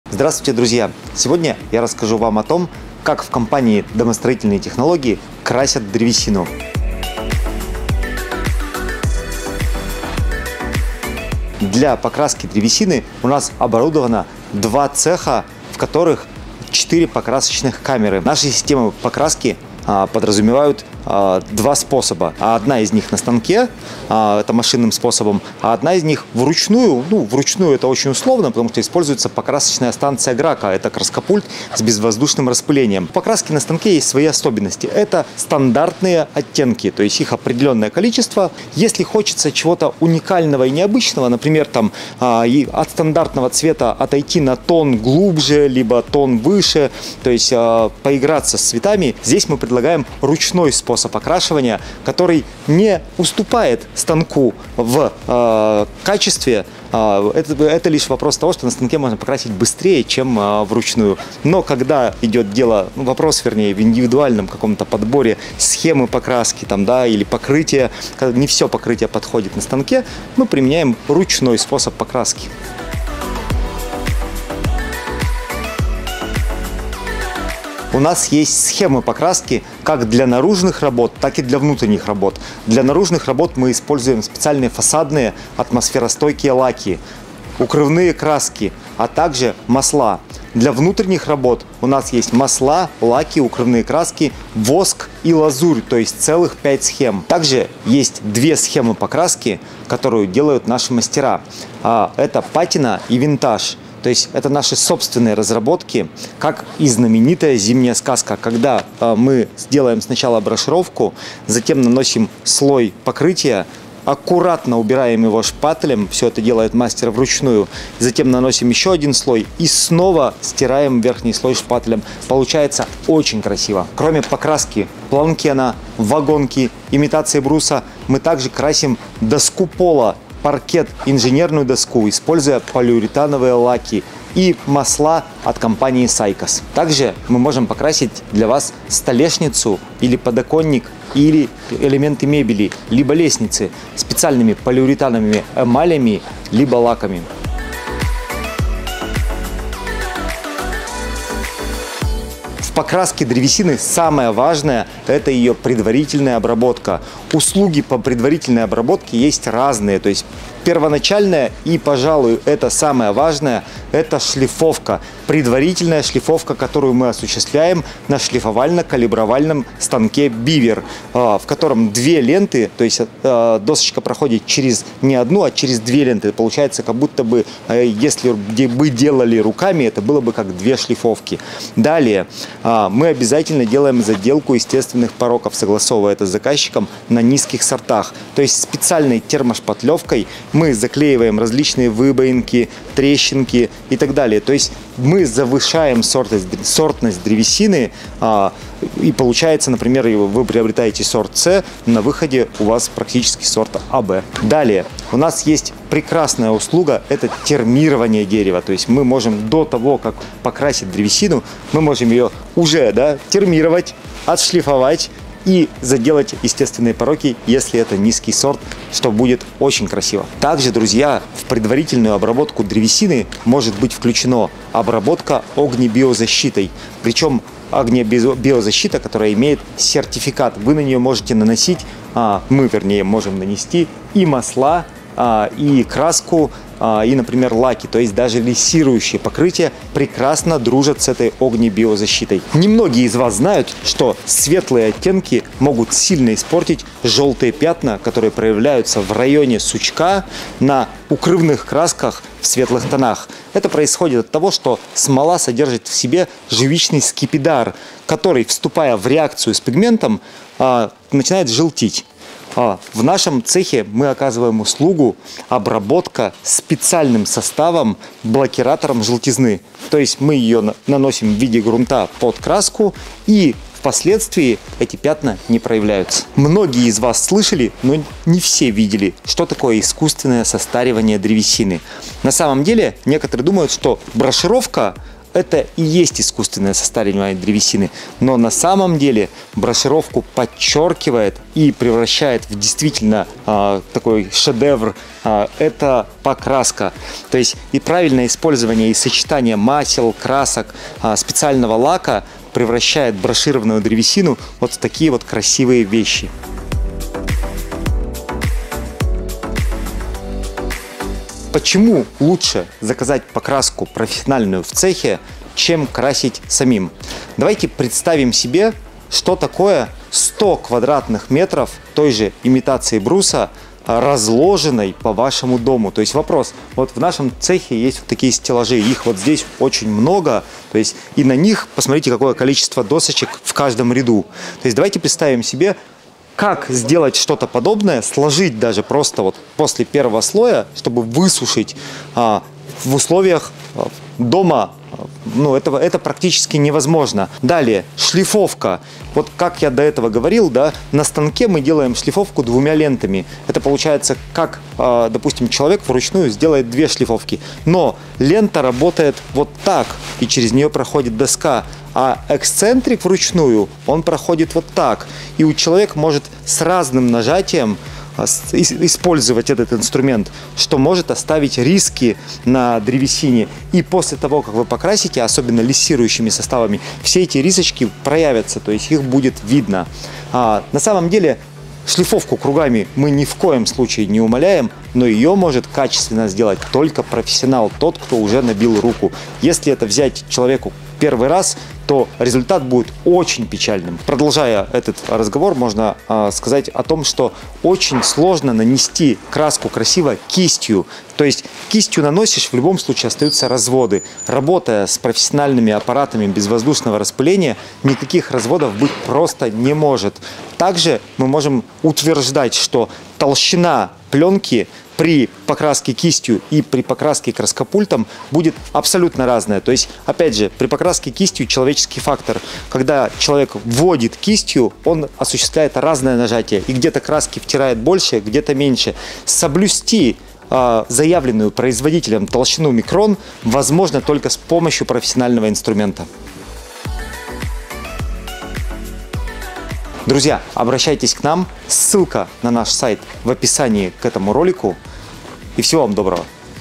Покраска древесины
Дорогие друзья! В этом ролике мы хотим вам рассказать, как в компании домостроительные технологии производится покраска древесины!

🕗Таймкоды:
00:00 Введение
00:06 Способы покраски
01:13 Схемы покраски
02:27 Предварительная обработка
05:06 Почему лучше заказать покраску в цехе
07:20 Заключение

© «Домостроительные технологии Павла Усманова»